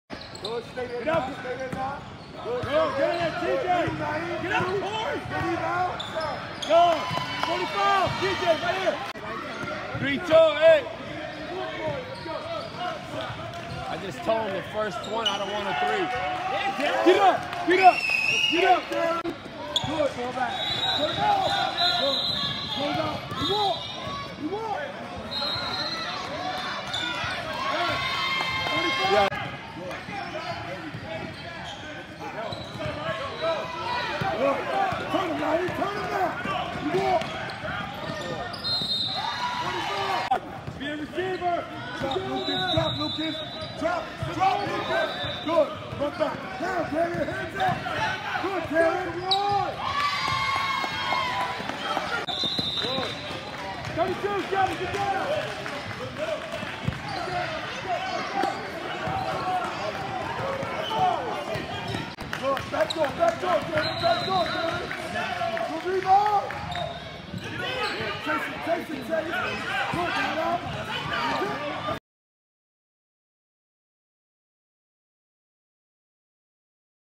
Blueprint MD defeats New Heights NY, 36-19 - Maryland Invitational Tournament
Maryland Invitational Tournament.